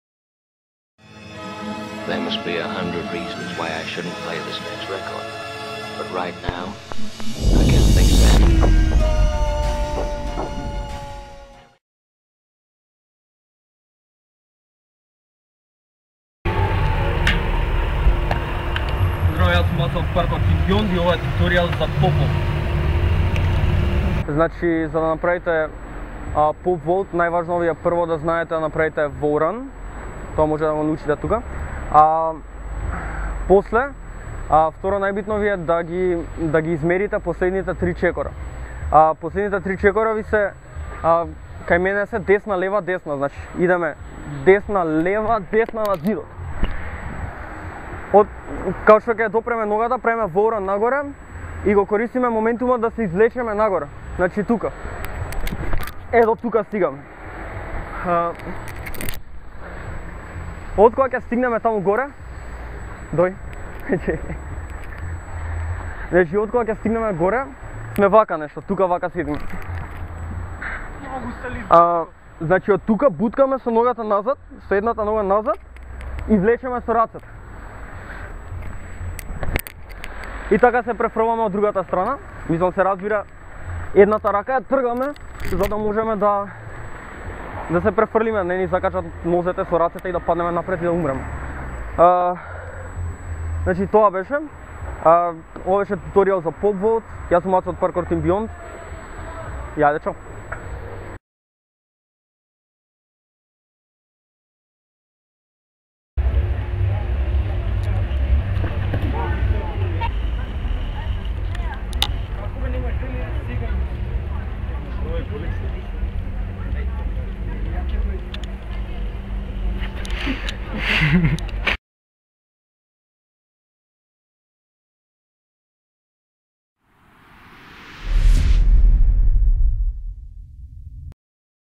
Objasnuvanje za Pop Vault
Објаснување за како правилно да изведеш Pop Vault на Македонски.